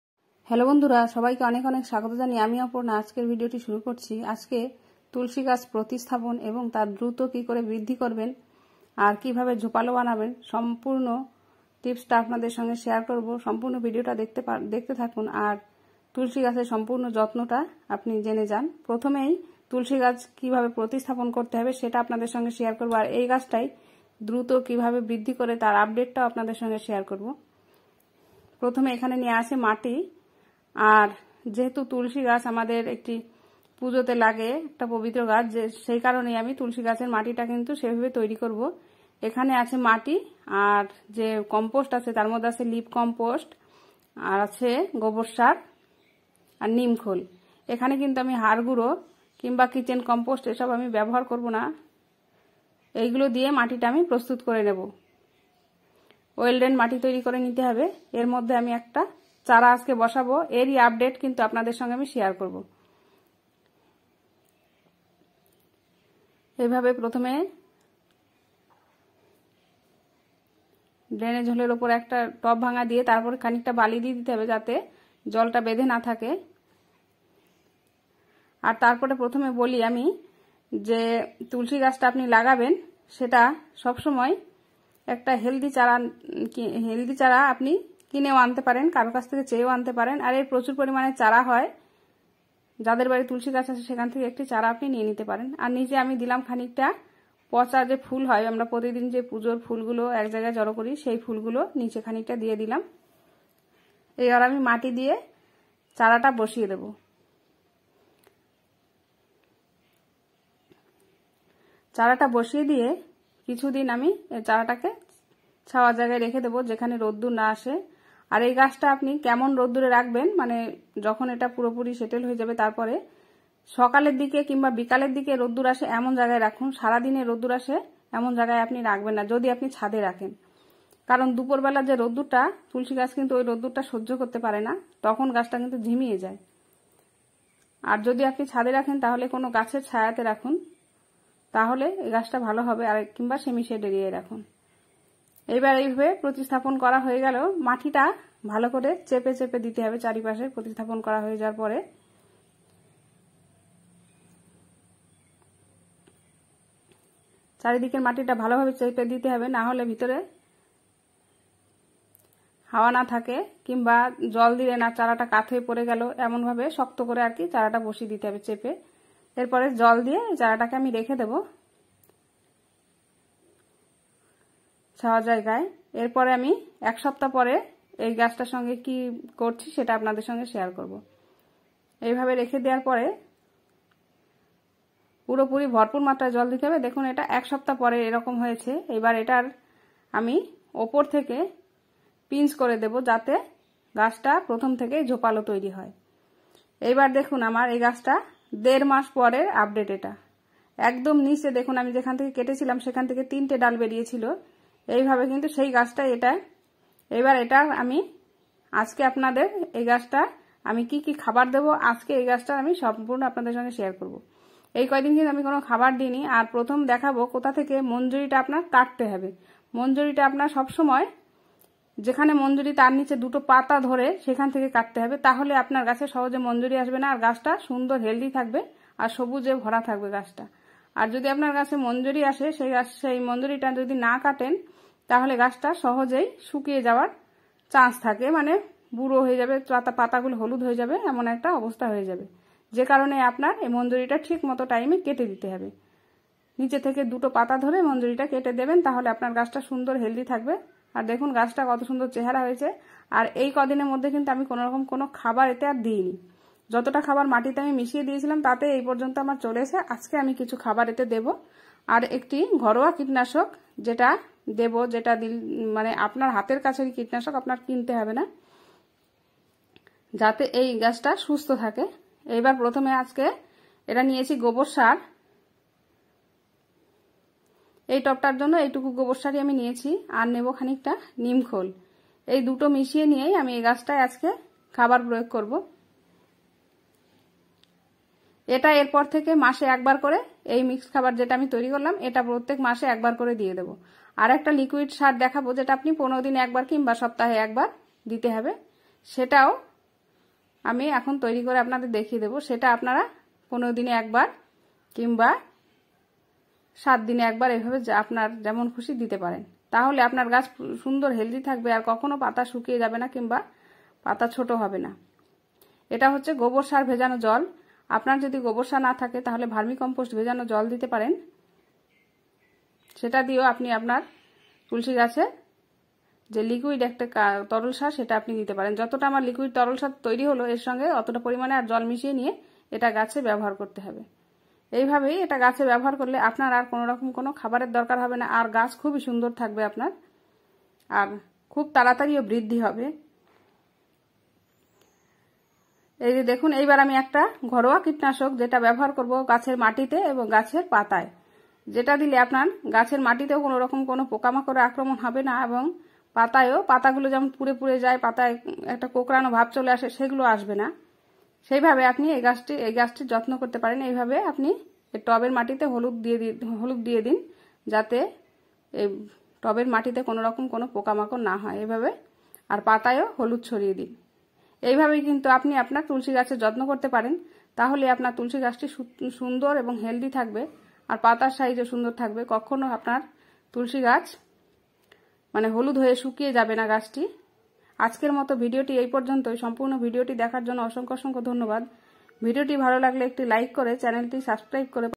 তুলসী গাছ দ্রুত বৃদ্ধির উপায় এবং সম্পূর্ণর যত্ন / Aparna Gardener
Tulsi plant care# Aparna Gardener

 তুলসী গাছের দ্রুত বৃদ্ধির উপায় / তুলসী গাছ কিভাবে যত্ন নেবেন/ তুলসী গাছ প্রতিস্থাপন / তুলসী গাছ কিভাবে দ্রুত ঝোপালো বানাবেন / আমার এই ভিডিওতে দেওয়া আছে তুলসী গাছে সম্পূর্ণ পরিচর্যা প্রতিস্থাপন এবং মাটি প্রস্তুতি কি খাবার দেবেন কি কি জৈব সার প্রয়োগ করবেন আর কিভাবে এর পোকামাকড়  দূর করবেন